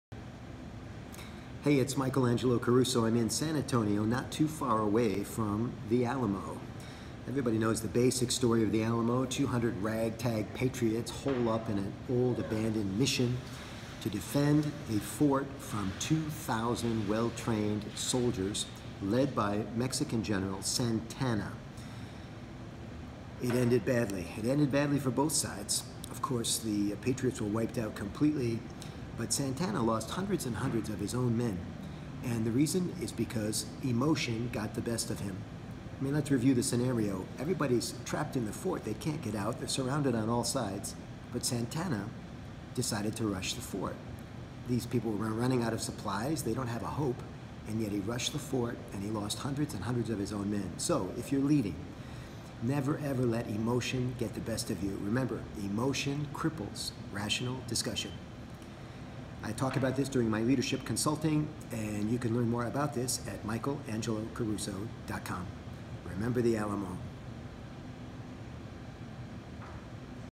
My version of The Alamo
I’m in San Antonio, Texas to speak at a leadership event.

Here’s the back story of what happened at the battle of The Alamo.

General Santana’s Mexican army of 2,000 soldiers had a band of 200 loosely organized patriots trapped in an old mission known as The  Alamo.

The Texas patriots included James Bowie, Davy Crockett and Colonel William Travis.

The people inside the mission were running out of supplies.  Santana could have waited them out, but he instead chose to storm the fort.

The defenders were wiped out, of course, and Santana lost nearly 600 men.

The reason he didn’t wait?  Ego and emotion.

Leaders, remember that emotion crippled rational thinking.

And remember the Alamo!